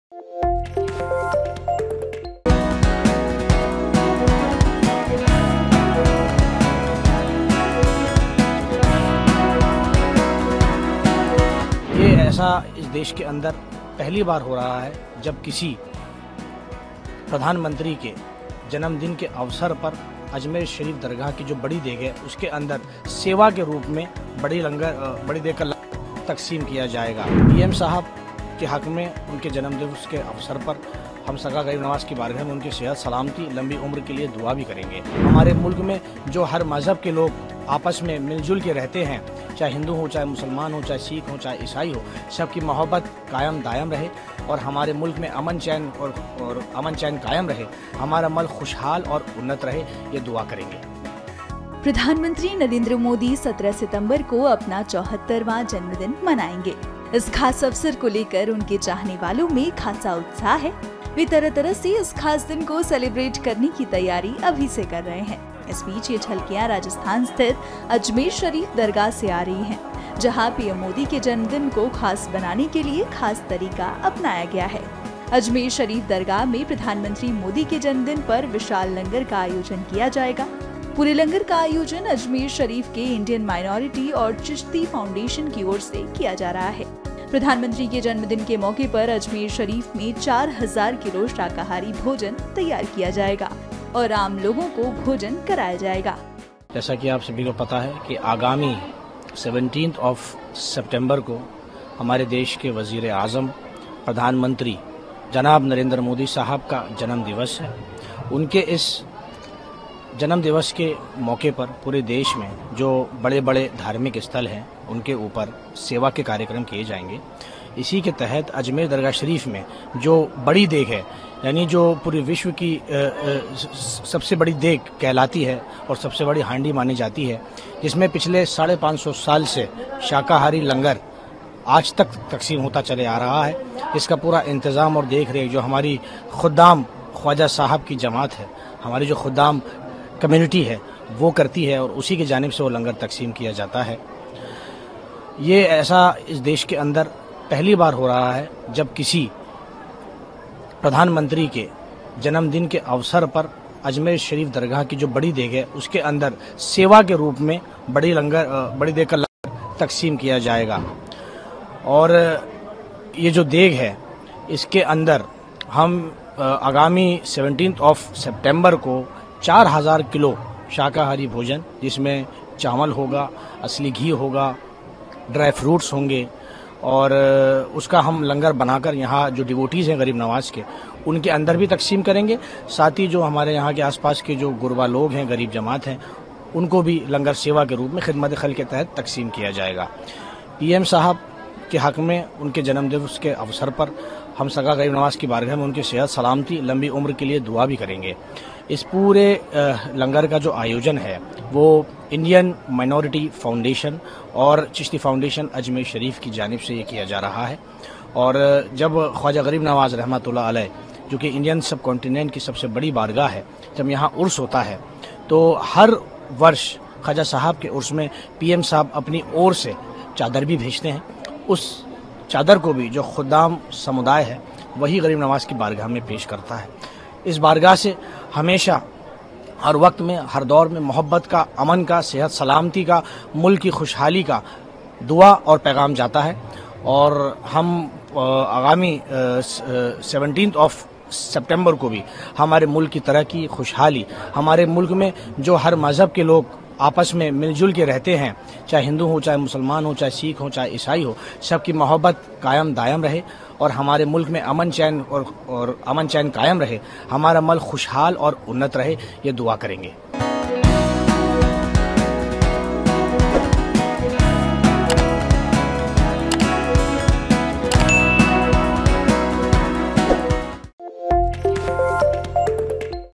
PM मोदी के जन्मदिन पर अजमेर शरीफ दरगाह में लंगर की विशेष व्यवस्था
प्रधानमंत्री नरेंद्र मोदी 17 सितंबर को अपना 74वां जन्मदिन मनाने वाले हैं. उनके चाहनेवाले इस दिन को खास बनाने के लिए विभिन्न तरीकों से तैयारी कर रहे हैं. अजमेर शरीफ दरगाह में भी इस मौके पर विशाल लंगर का आयोजन होगा.